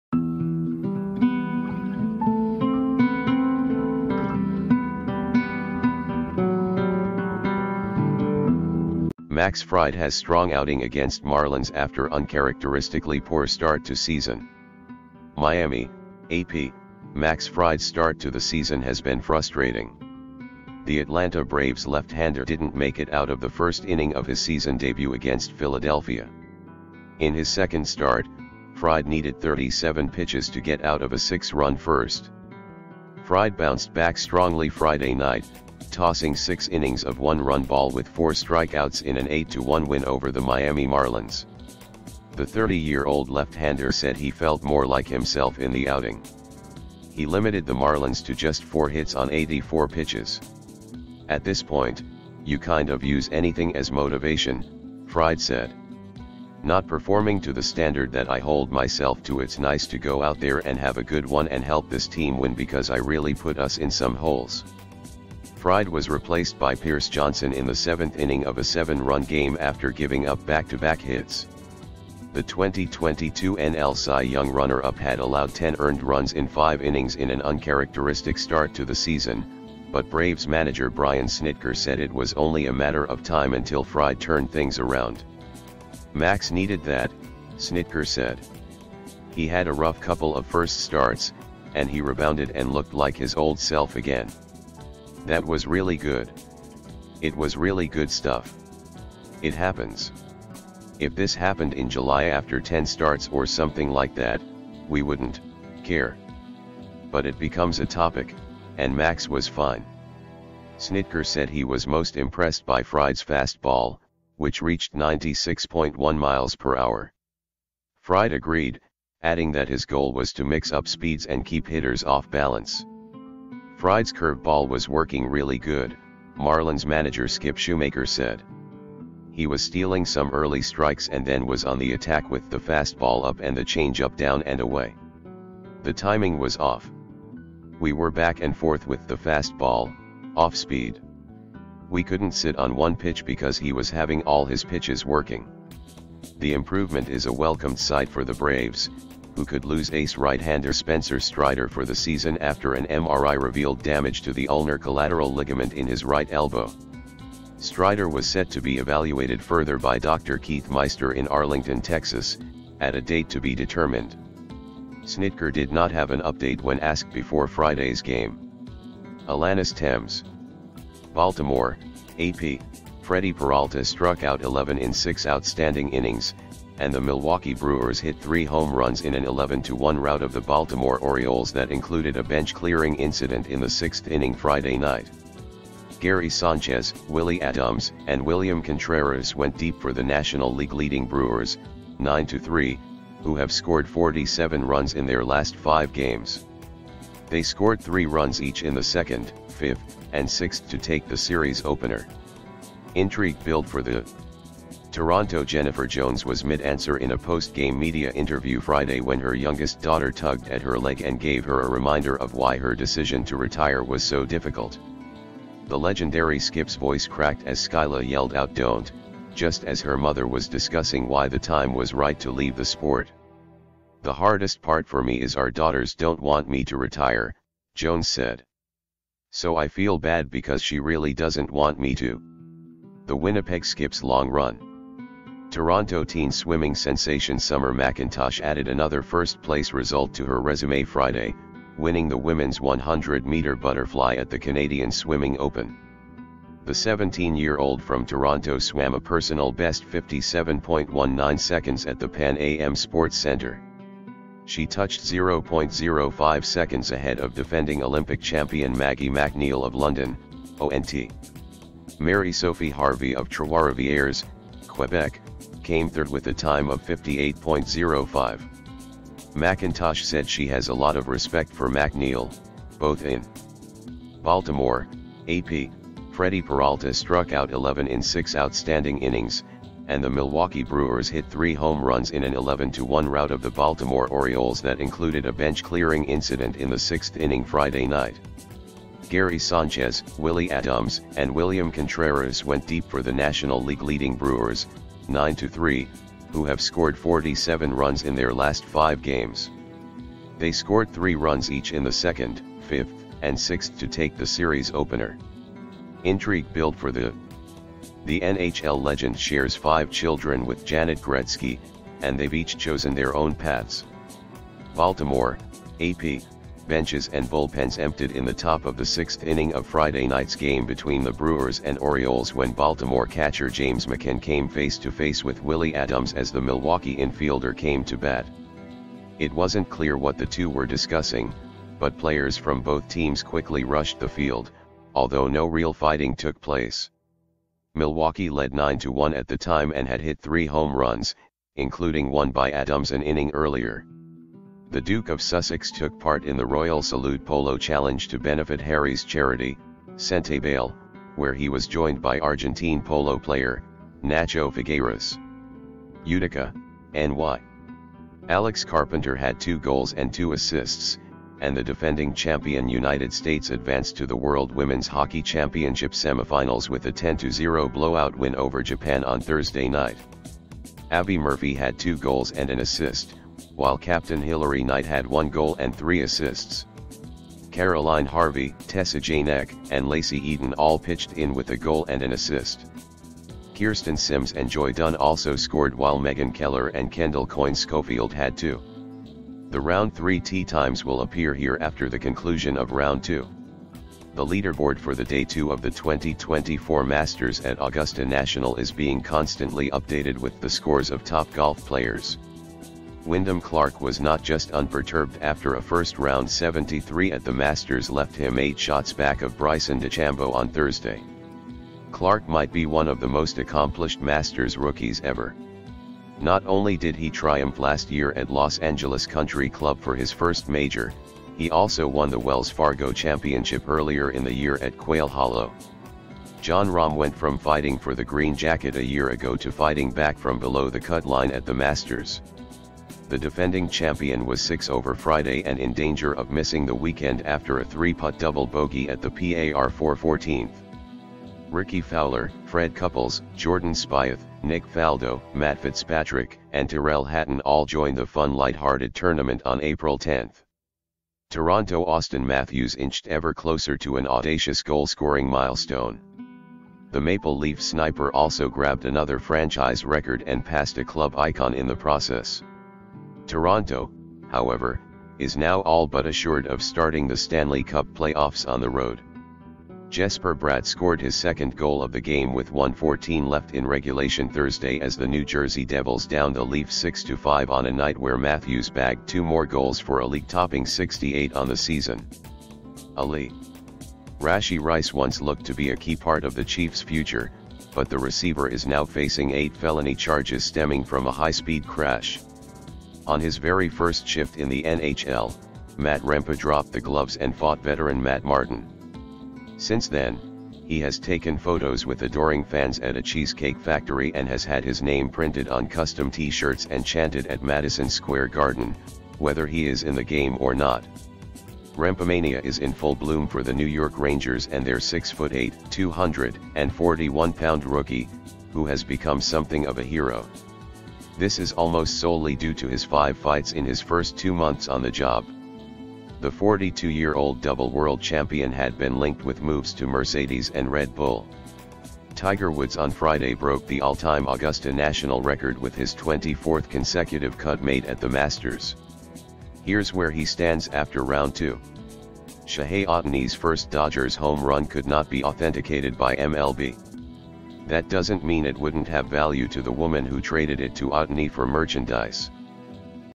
🔴Max Fried has strong outing against Marlins after uncharacteristically poor start to season🔴
MIAMI (AP) — Max Fried's start to the season has been frustrating.

The Atlanta Braves left-hander didn't make it out of the first inning of his season debut against Philadelphia. In his second start, Fried needed 37 pitches to get out of a six-run first.

Fried bounced back strongly Friday night, tossing six innings of one-run ball with four strikeouts in an 8-1 win over the Miami Marlins. The 30-year-old left hander said he felt more like himself in the outing. He limited the Marlins to just four hits on 84 pitches.

“At this point, you kind of use anything as motivation,” Fried said. "Not performing to the standard that I hold myself to — it's nice to go out there and have a good one and help this team win because I really put us in some holes."

Fried was replaced by Pierce Johnson in the seventh inning of a seven-run game after giving up back-to-back hits.

The 2022 NL Cy Young runner-up had allowed 10 earned runs in five innings in an uncharacteristic start to the season, but Braves manager Brian Snitker said it was only a matter of time until Fried turned things around.

“Max needed that,” Snitker said. “He had a rough couple of first starts, and he rebounded and looked like his old self again. That was really good. It was really good stuff. It happens. If this happened in July after 10 starts or something like that, we wouldn’t (care). But it becomes a topic, and Max was fine.”

Snitker said he was most impressed by Fried's fastball, which reached 96.1 mph. Fried agreed, adding that his goal was to mix up speeds and keep hitters off balance.

“Fried’s curve ball was working really good," Marlins manager Skip Schumaker said. "He was stealing some early strikes and then was on the attack with the fastball up and the changeup down and away.